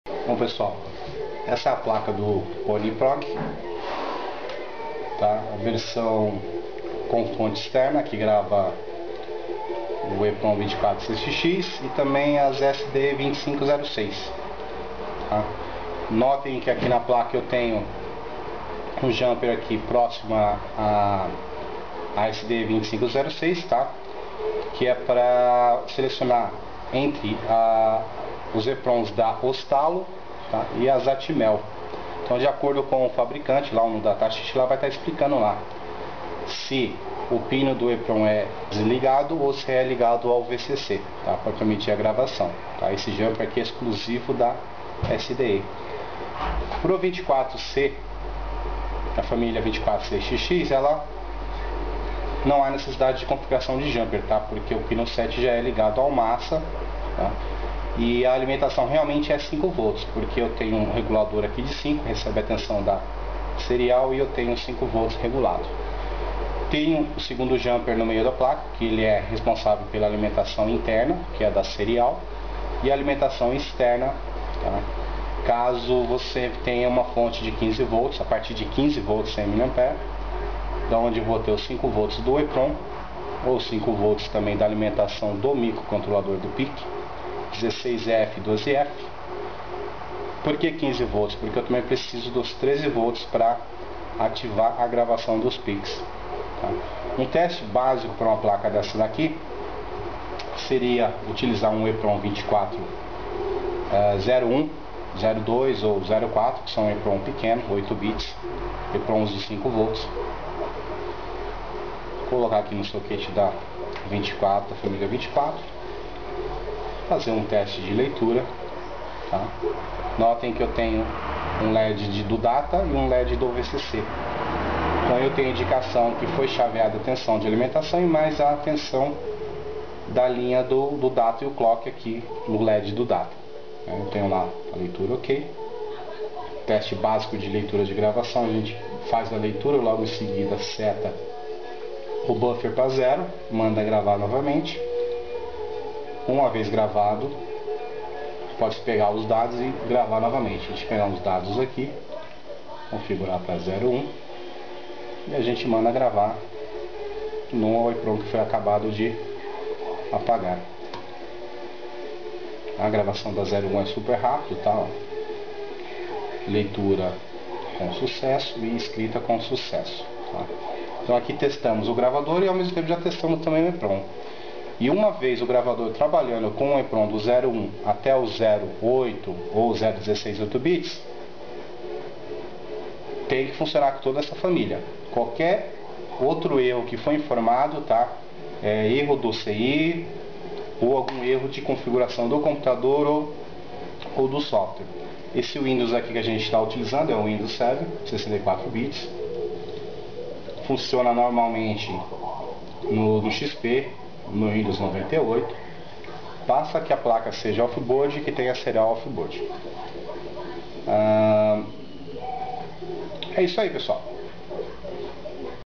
ponyprog_1.avi
Demonstração do gravador de eeprom ponypro com software ponyprog